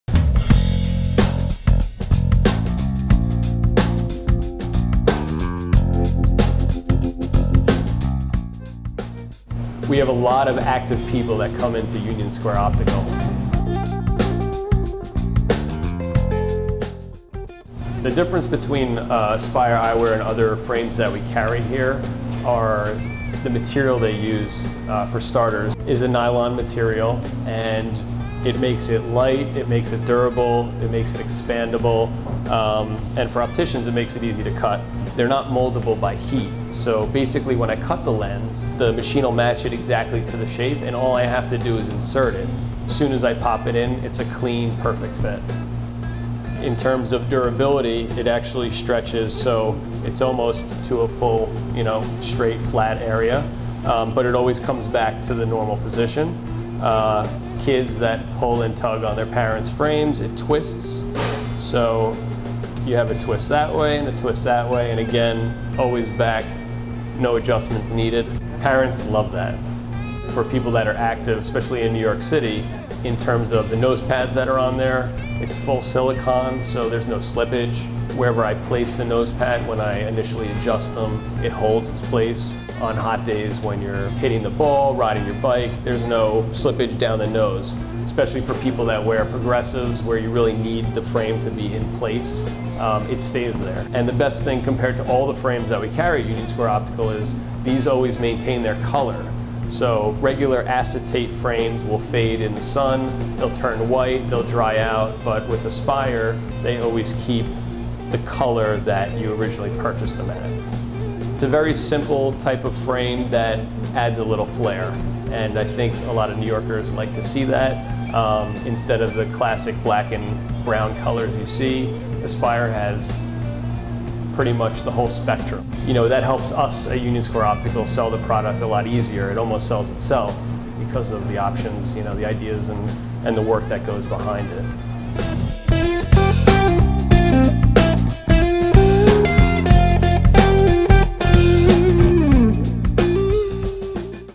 Focus on New York: Aspire Eyewear at Union Square Optical
See Aspire Eyewear at NYC's Union Square Optical. Here, optician Marc Rosen discusses the benefits of the collection including its lightweight durability stemming from the material with which the frames are created, SDN-4.

To see the entire Aspire collection or locate a retailer near you,